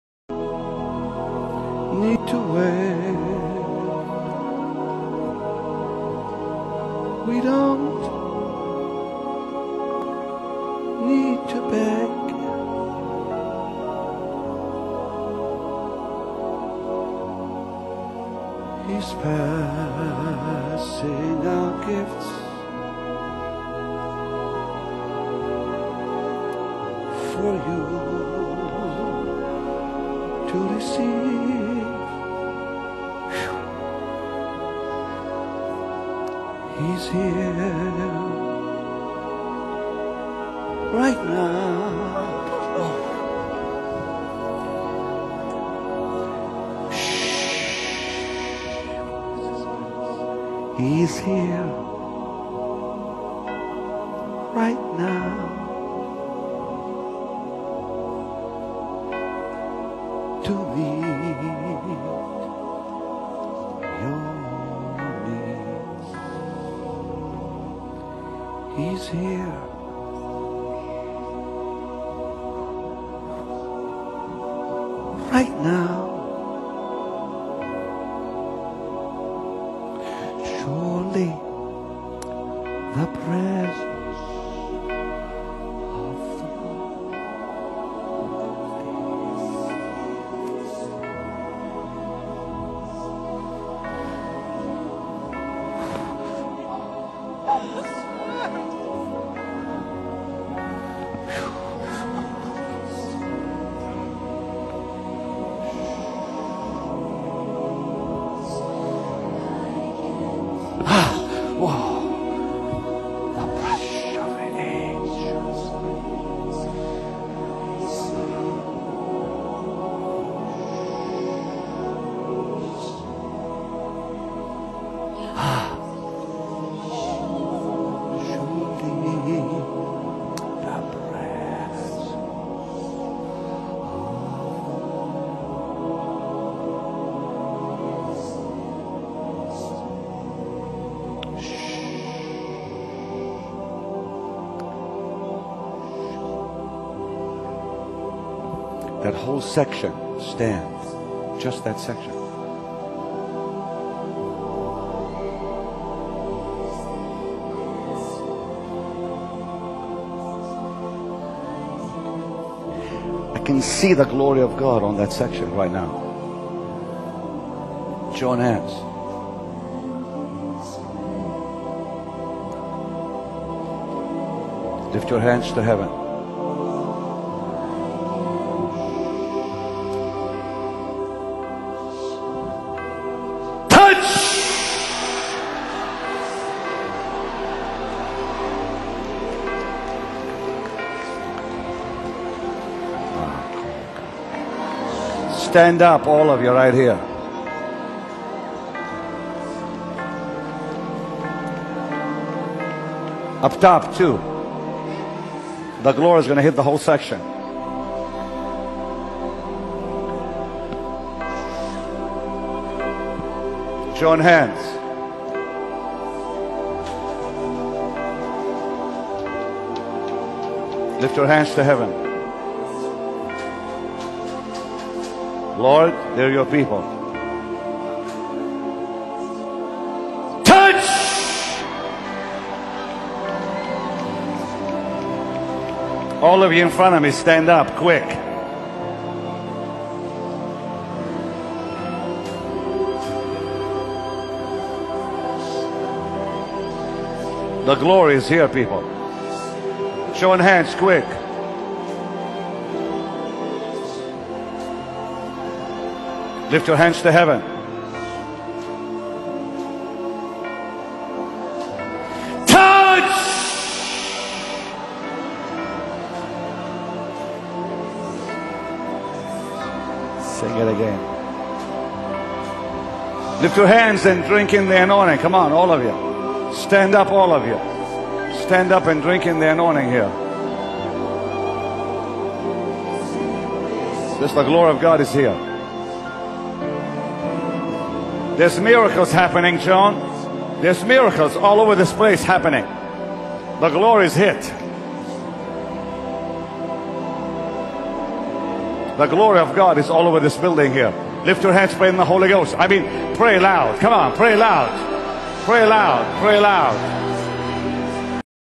The Holy Ghost Touch. Powerful Anointing Service By Pastor Benny Hinn.
When you have the anointing of God  you will never remain the same. You will be transformed, and become a useful vessel which transforms lives. Anointing has a great significance as it symbolizes the Lord's favour. 1 John 2:27